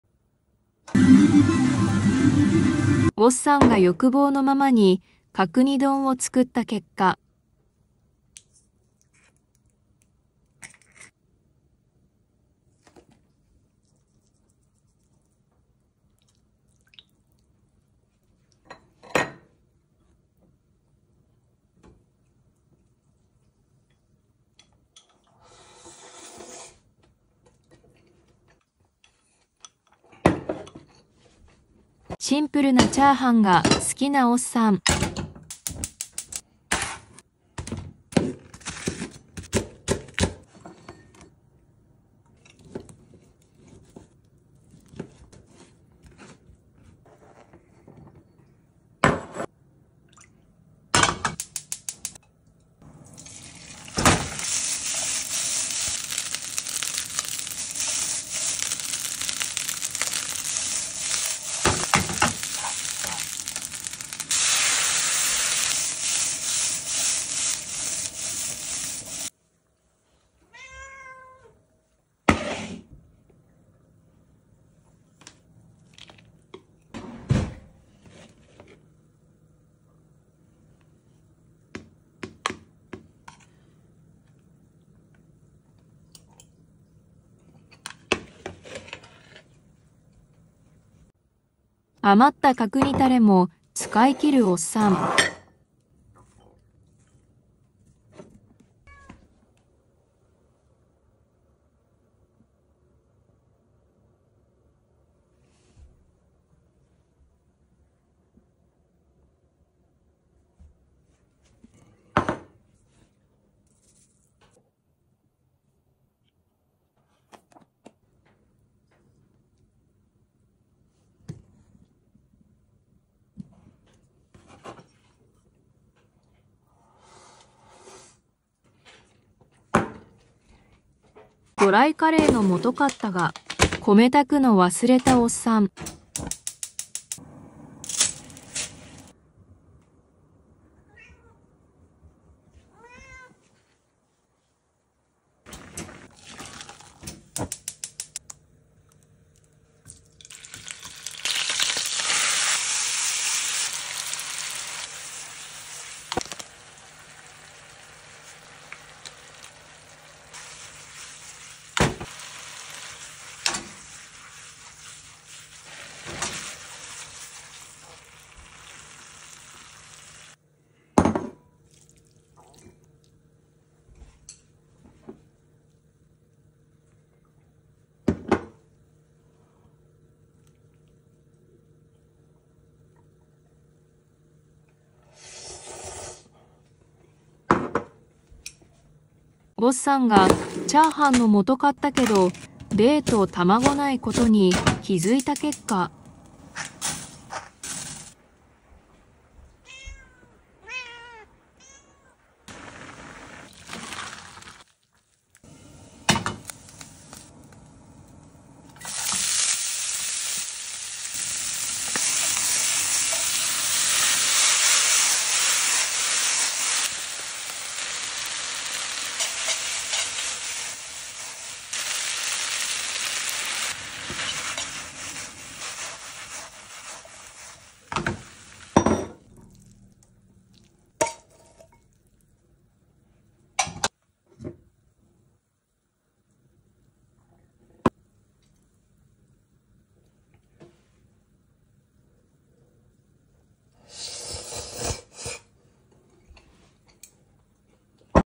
【飯テロ】2023年5月のオッサン料理5品④とたまに猫 5 Ossan dishes for May 2023 ④ and occasional cats
▼目次▼
0:00 角煮丼
0:32 自家製角煮のシンプルな炒飯
1:31 角煮タレネギマヨ丼
2:10 ドライカレーの素パスタ
3:09 炒飯の素パスタ

料理,貧乏飯,アレンジ,飯テロ,小ネギ,角煮,明太子,マヨネーズ,パスタ,ドライカレー,炒飯,豚バラ,